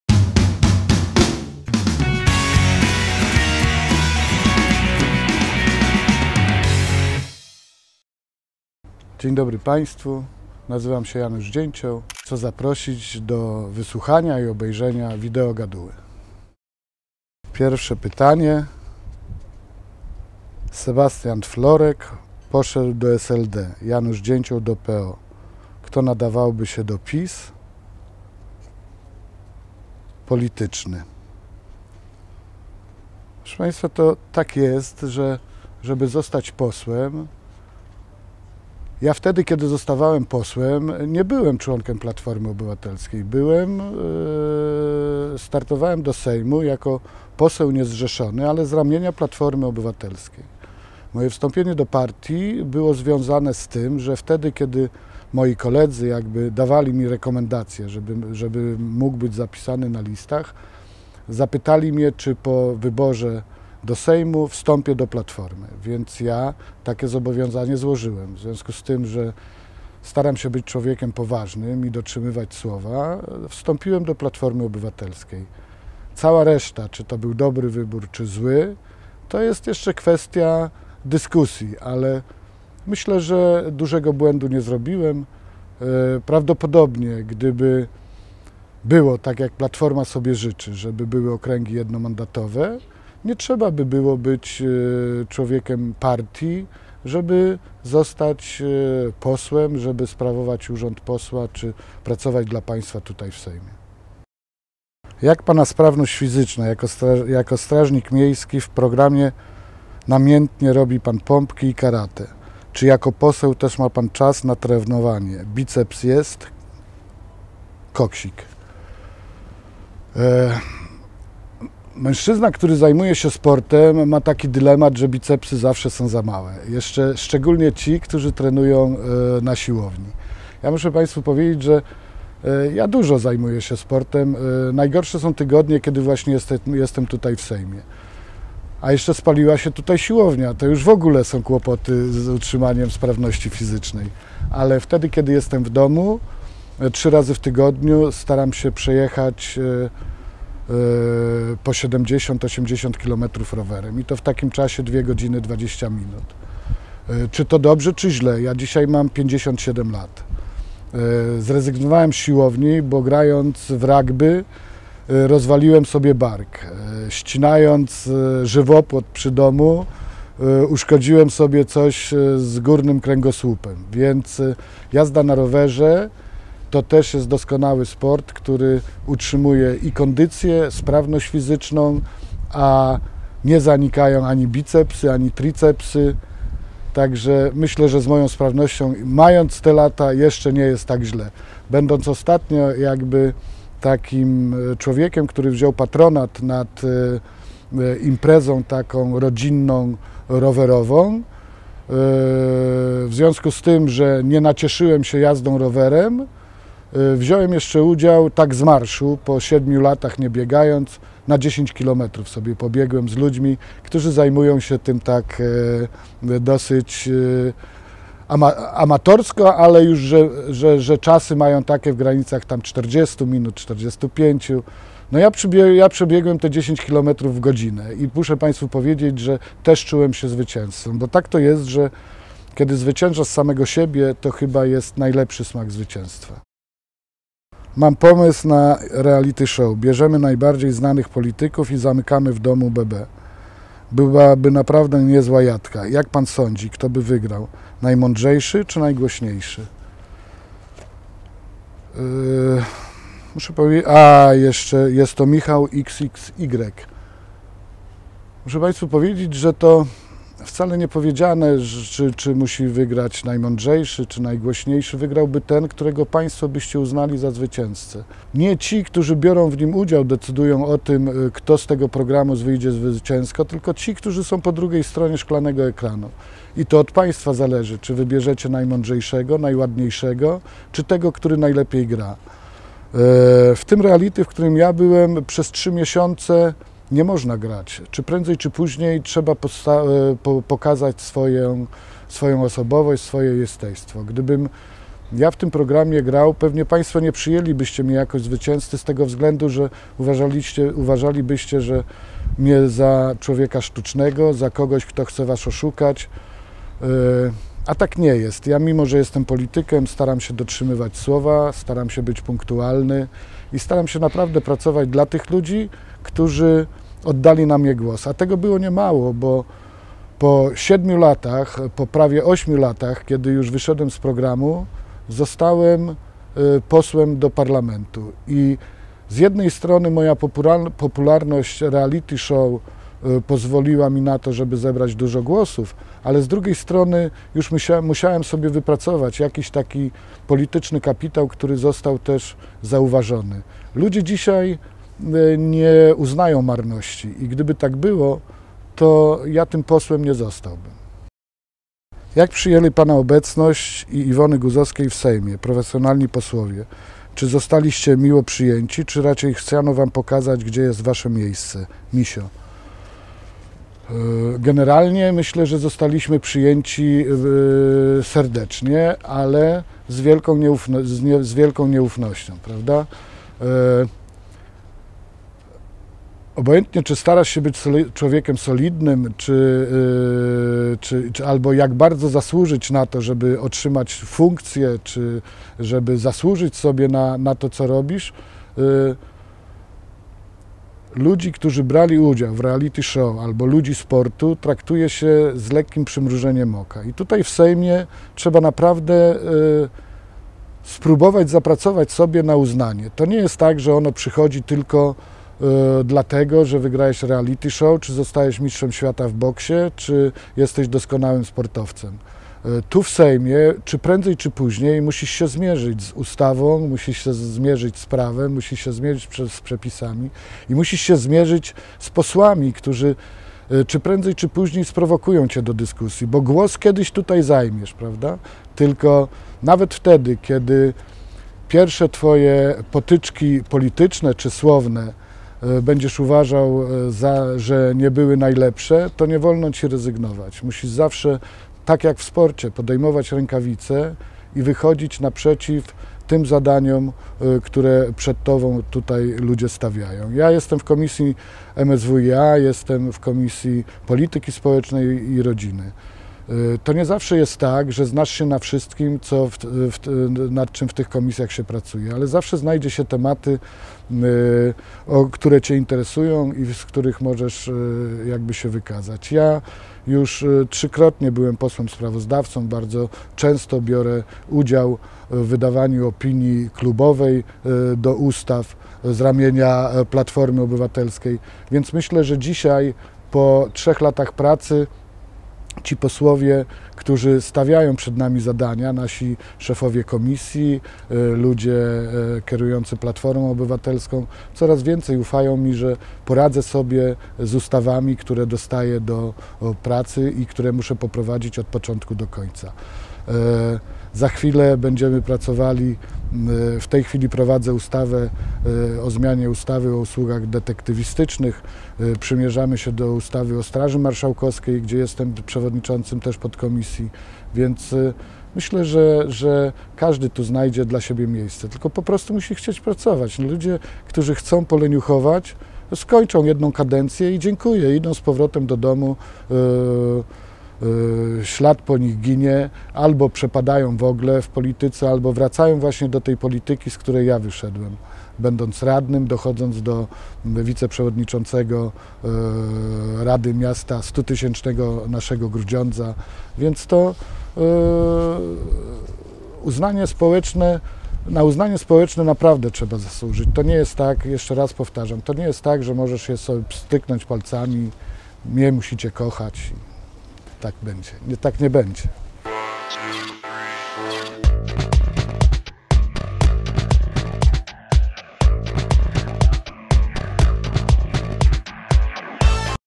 Videogaduła, odc. 5: Janusz Dzięcioł, cz. I/III
Janusz Dzięcioł- polityk PO, poseł na Sejm VI kadencji. Pełnił funkcję komendanta Straży Miejskiej w Świeciu, radnego i wiceprzewodniczącego rady miejskiej w Grudziądzu (2002- 07). W 2001 roku wziął udział w pierwszej edycji "Big Brothera", nadawanej w telewizji TVN, wygrywając ją. Zagrał samego siebie w filmie Jerzego Gruzy  zatytułowanym "Gulczas, a jak myślisz...". W 2007r. uzyskał mandat poselski, startując w wyborach parlamentarnych z listy PO w okręgu toruńskim. Później został członkiem Platformy Obywatelskiej.

Zapraszamy do Videogaduły z Januszem Dzięciołem: cz. II cz. III

W cz. I Videogaduły poseł zdradza:
- Kto nadawałby się do PiS?
- Czy wciąż ma czas na trenowanie?
- Czy pomysł na Big Brother'a z politykami sprawdziłby się?
- Jak przyjęto w Sejmie obecność jego i Iwony Guzowskiej?